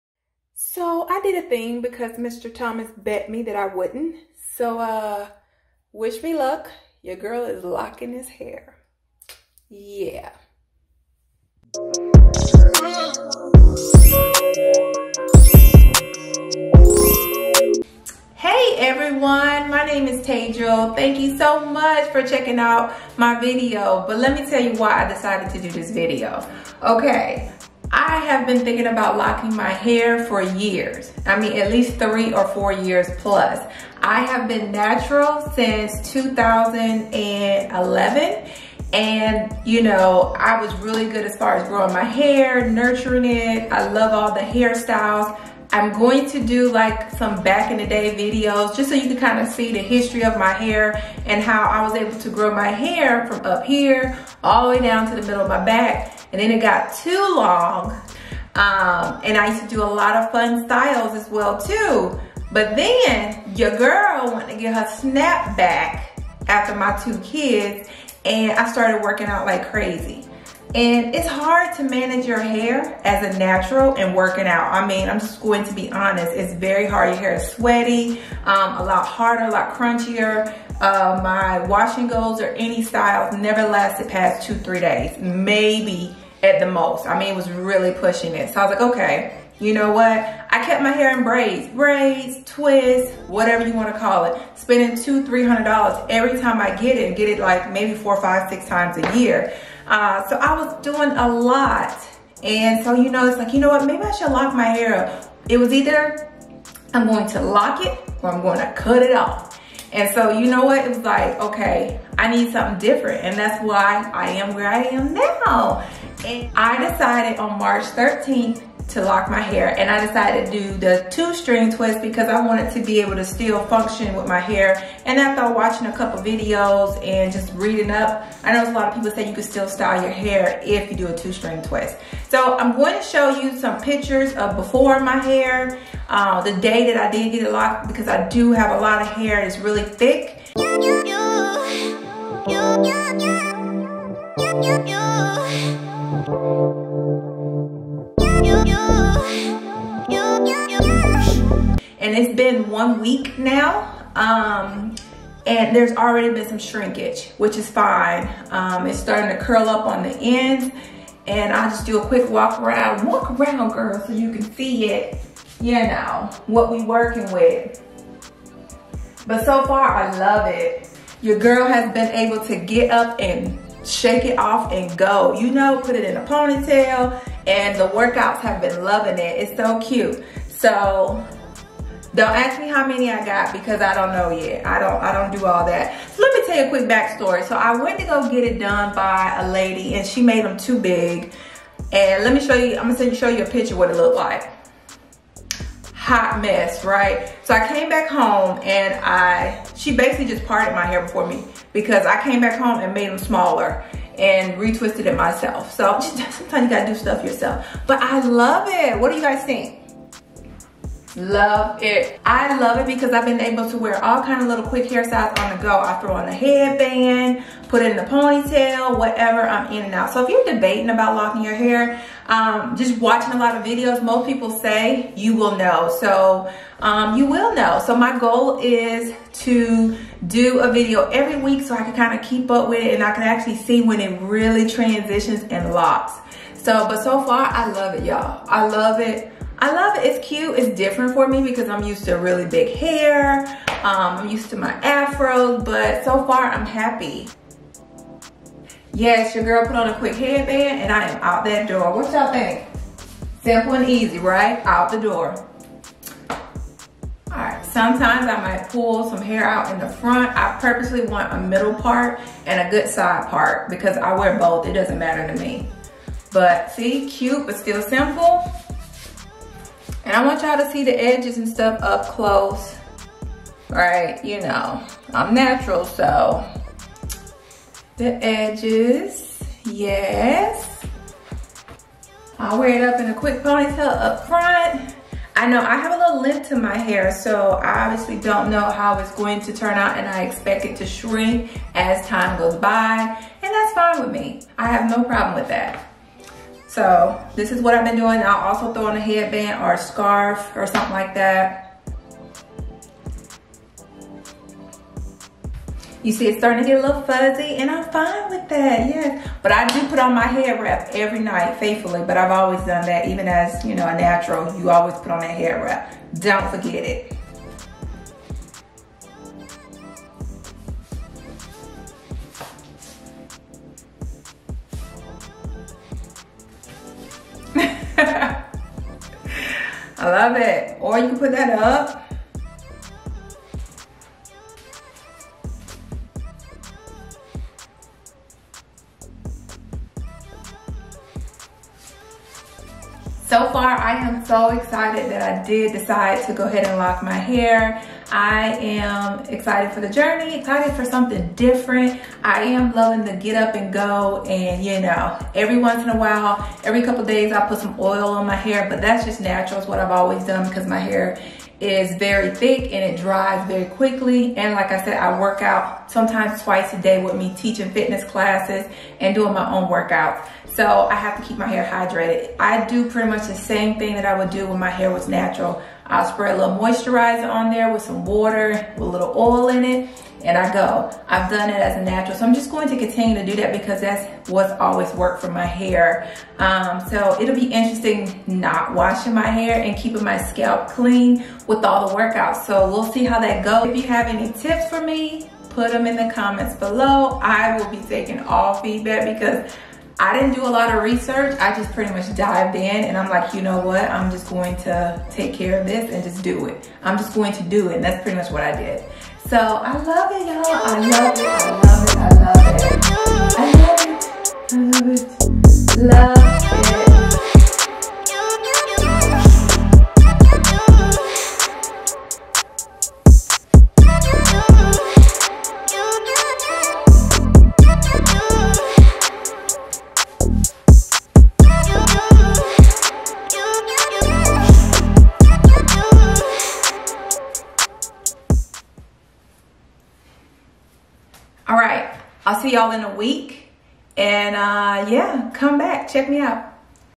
OMG I Finally Loc'd my Hair ▪︎》2 Strand Twists Starter Locs
Hey friends heyyyy! So we are here and I have finally loc'd my hair after debating to do it for so many years.